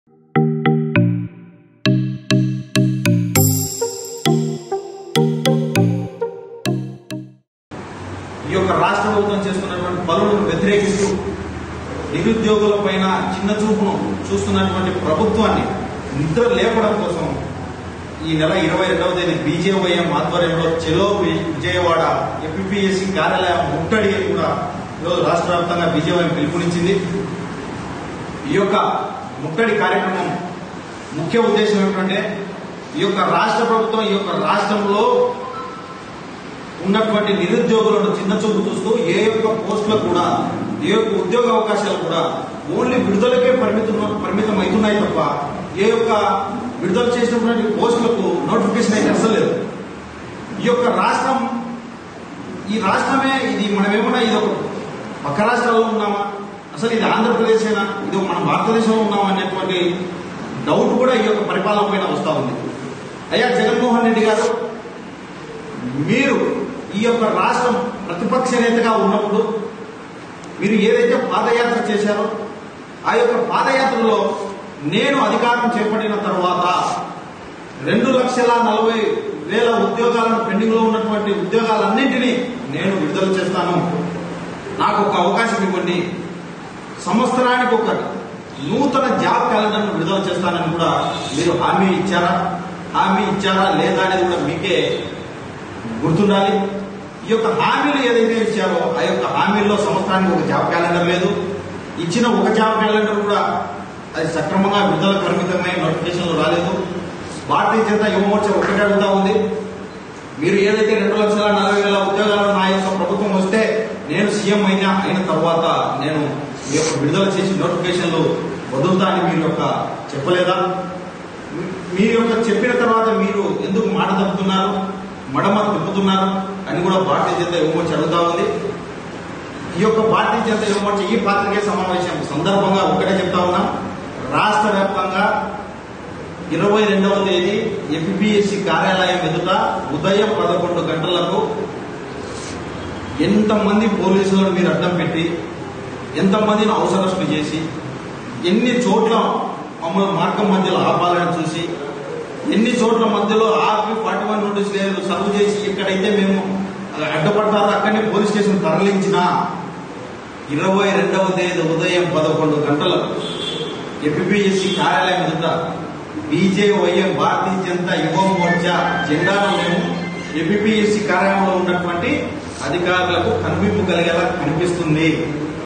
Kadapa BJYM President Sravan Kumar Reddy About Ycp Govt | Cm Ys Jagan | Ap Jobs | Krishna Talks
Discussions And Analysis On Andhra Pradesh Political , Current affairs, Exclusive Interviews..